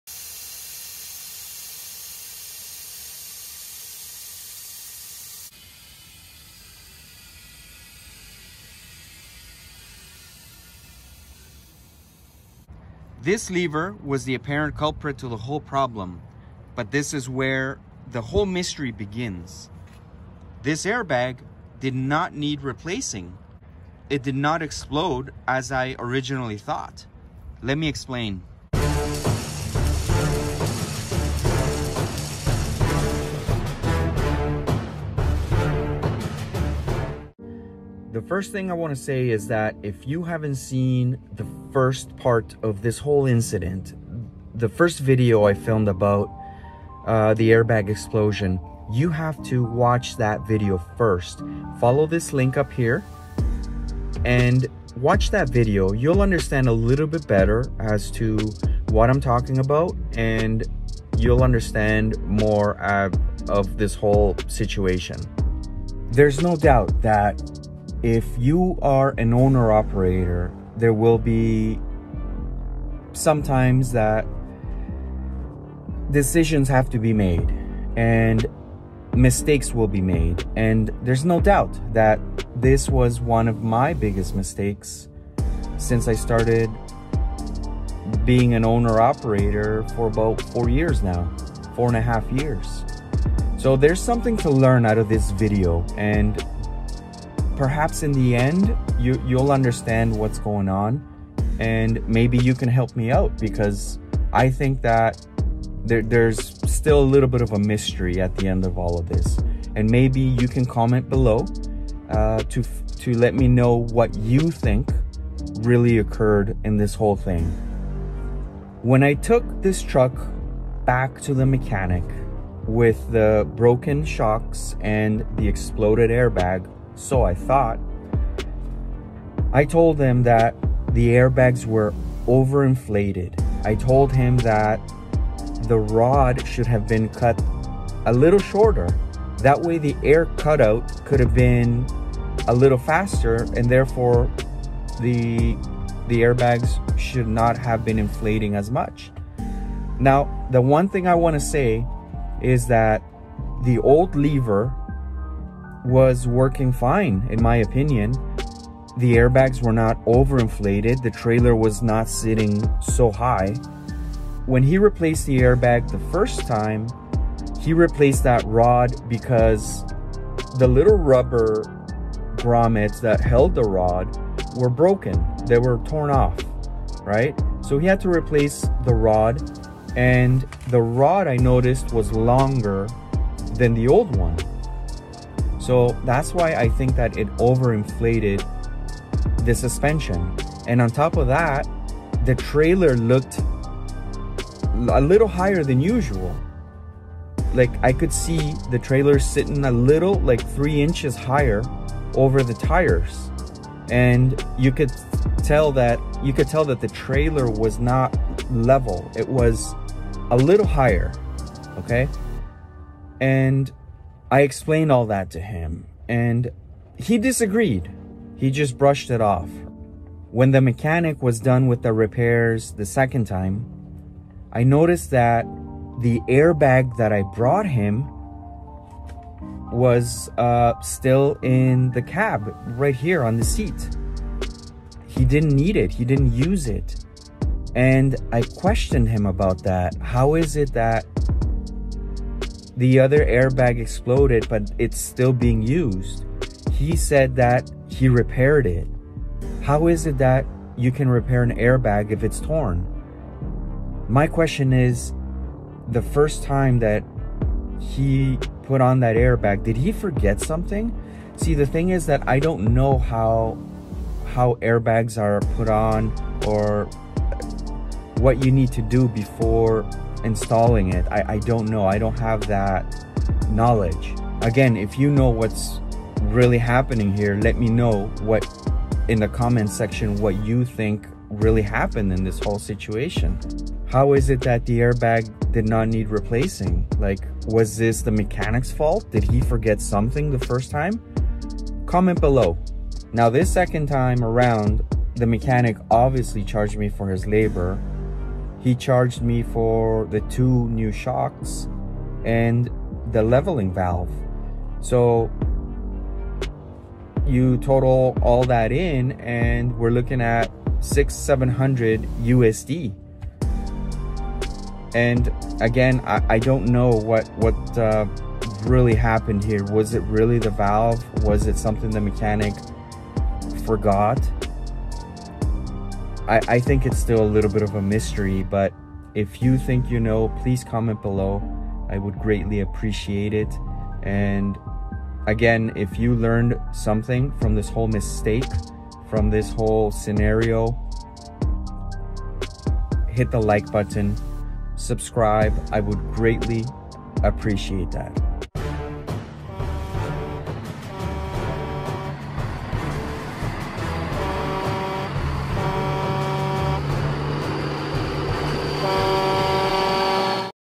Suspension air bag did NOT explode. My mechanic fixed it! How?
I don't wan't to make excuses and try to blame my mechanic for this failure, but there's something that's not adding up in all this story. It was definitely my fault for driving without any air in the suspension and then breaking my shocks in the first place but what do you think happened here?